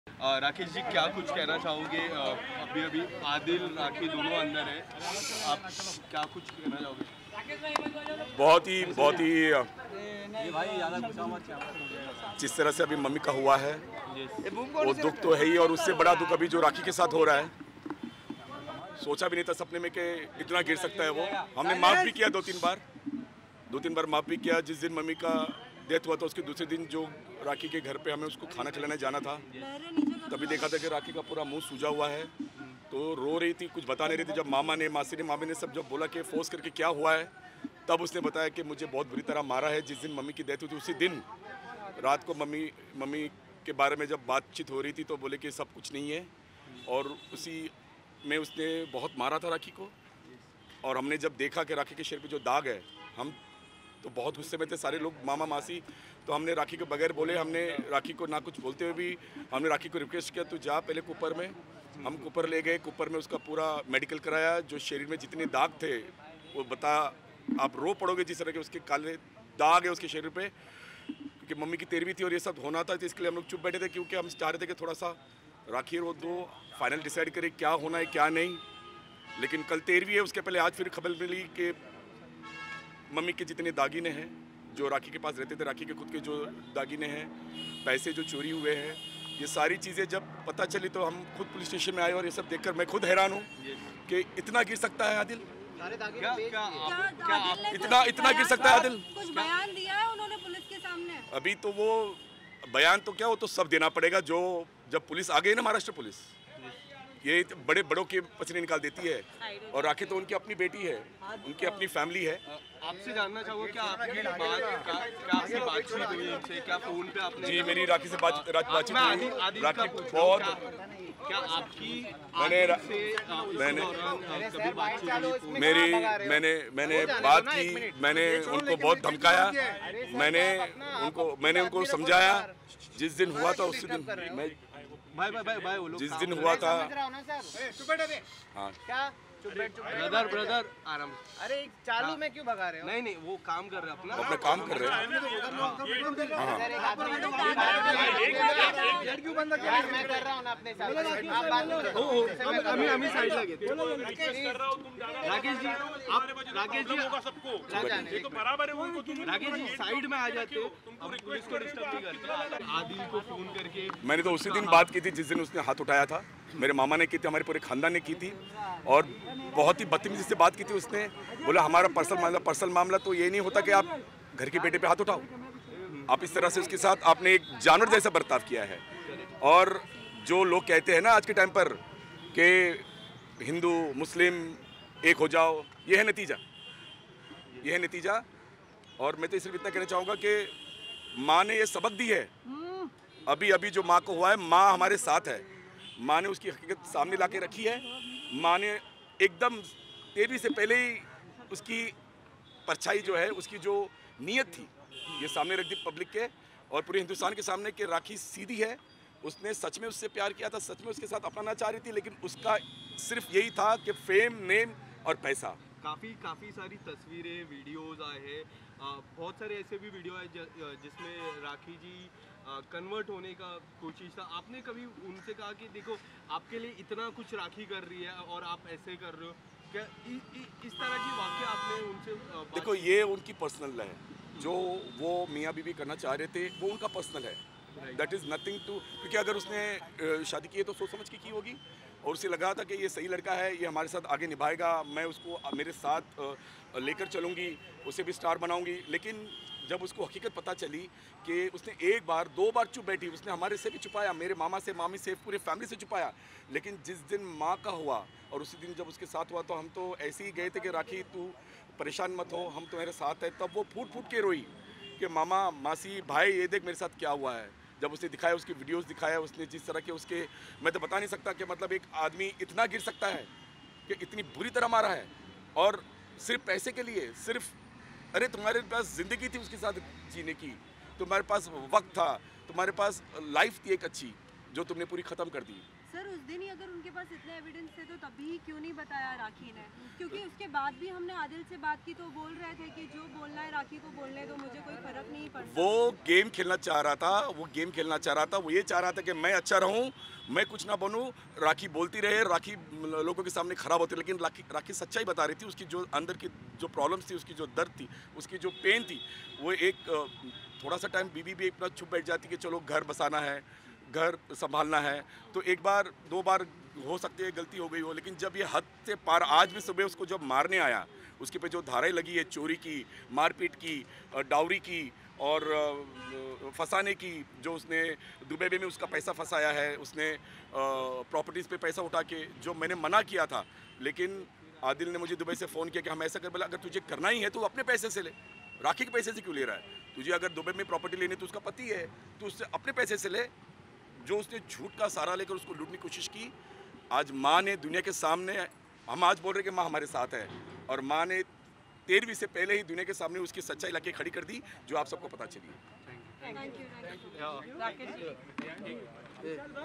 Byte Of Rakhi Sawant Brother Rakesh Sawant At Oshiwara Police Station | MS shorts
Byte Of Rakhisawant Brother Rakeshsawant At Oshiwara Police Station